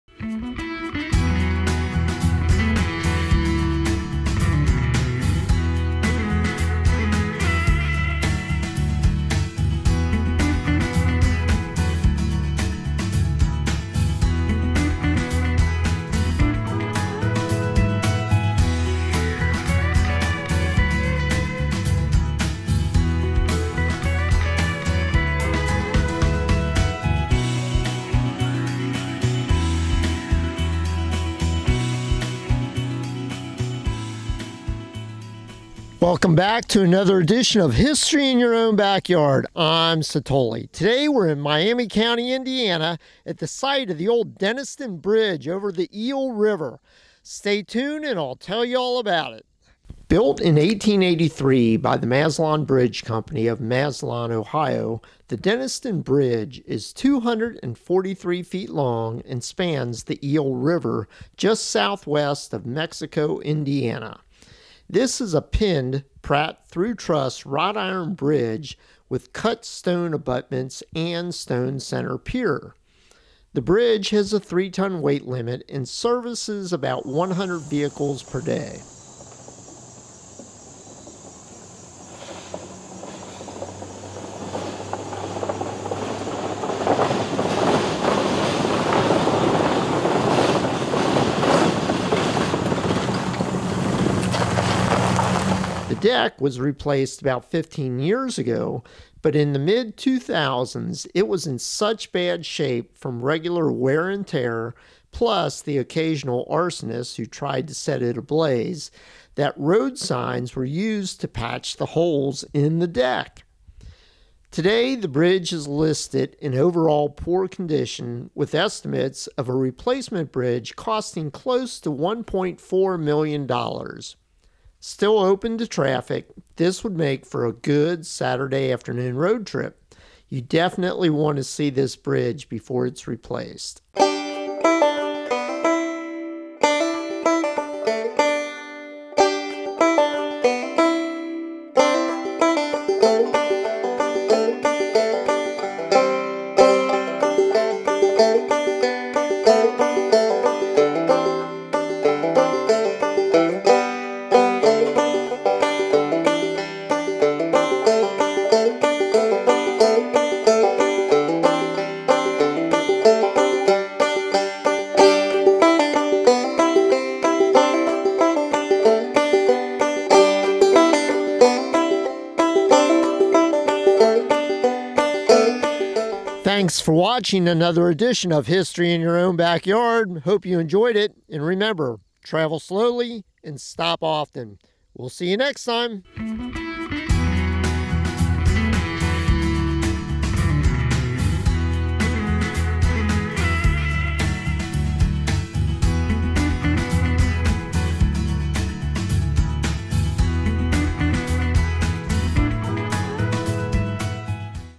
The Denniston Bridge, Mexico, Indiana
Built  in  1883  by  the  Massillon  Bridge  Company  of  Massillon,  Ohio,  the  Denniston  bridge  is  243  feet  long  and  spans  the  Eel  River  just  southwest  of  Mexico,  Indiana.  This  is  a  pinned  Pratt  through  truss  Wrought  Iron  Bridge  with  cut  stone  abutments  and  stone  center  pier.  The  bridge  has  a  3  ton  weight  limit  and  services  about  100  vehicles  per  day.

The  deck  was  replaced  about  15  years  ago  but  in  the  mid  2000s  it  was  in  such  bad  shape  from  regular  wear  and  tear  plus  the  occasional  arsonists  who  tried  to  set  it  ablaze,  that  road  signs  were  used  to  patch  the  holes  in  the  deck!

Today  the  bridge  is  listed  in  overall  Poor  condition  with  estimates  of  a  replacement  bridge  costing  close  to  1.4  million  dollars.

Still  open  to  traffic,  this  would  make  for  a  good  Saturday  afternoon  road  trip.  You  definitely  want  to  see  this  bridge  before  it’s  replaced.